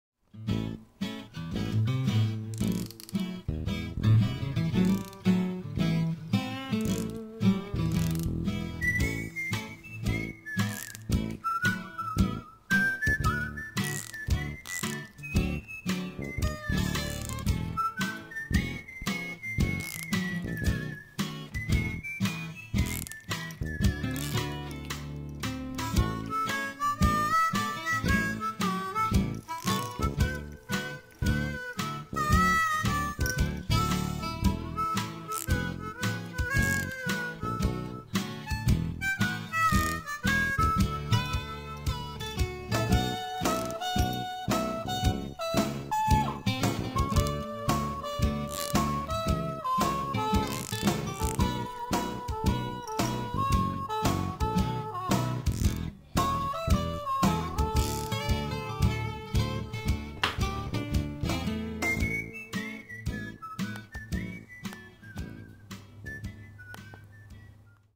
Simon the Cat
Siamese Cat with funny reaction the sound of a comb.